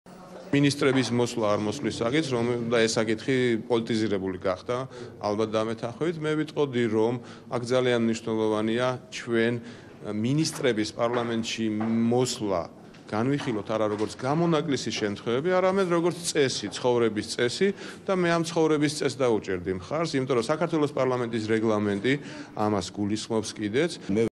გიგლა აგულაშვილის კომენტარი
გიგლა აგულაშვილის კომენტარი პრეზიდენტის პარლამენტში ვიზიტის შესახებ.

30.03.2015